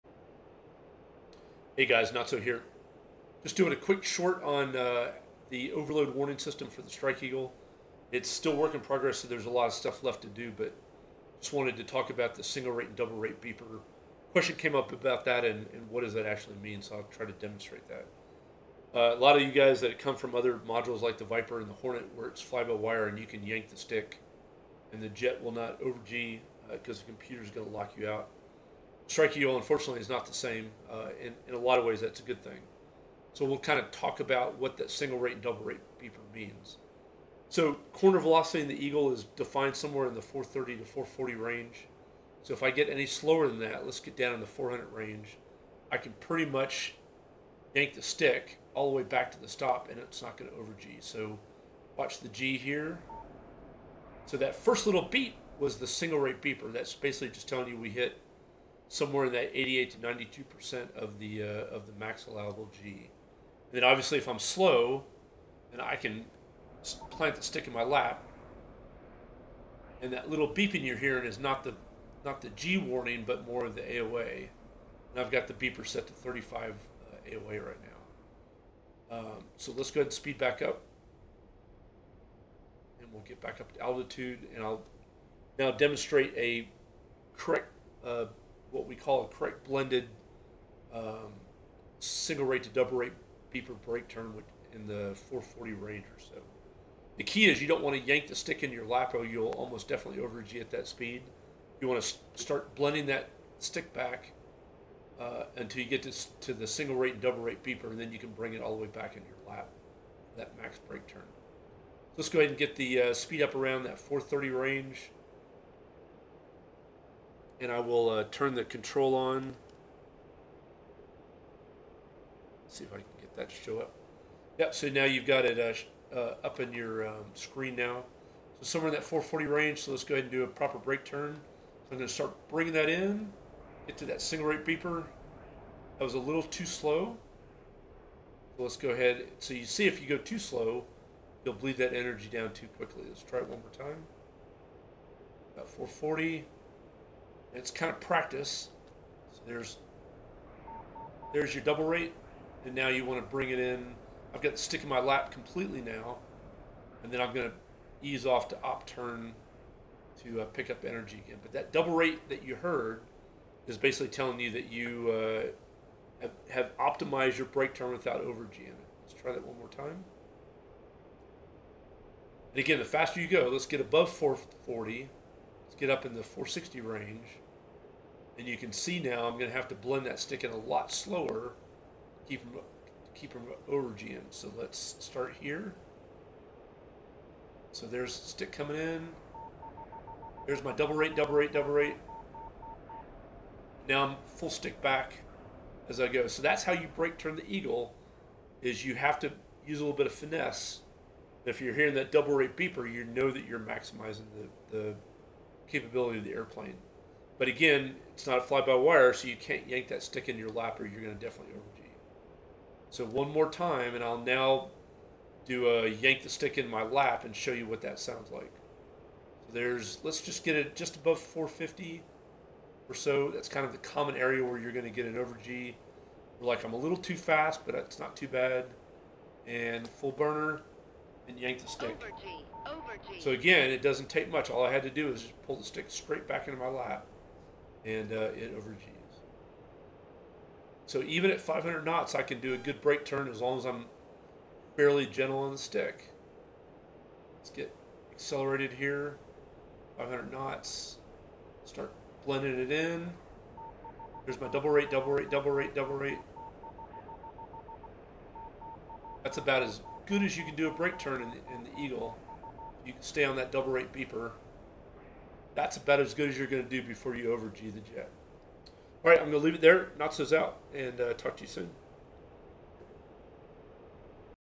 F-15E OWS (Overload warning system) - Single rate and double rate beeper
This is a quick vid on a proper break turn in the Eagle without Over-Ging the jet using the Single rate and Double rate beeper audible warnings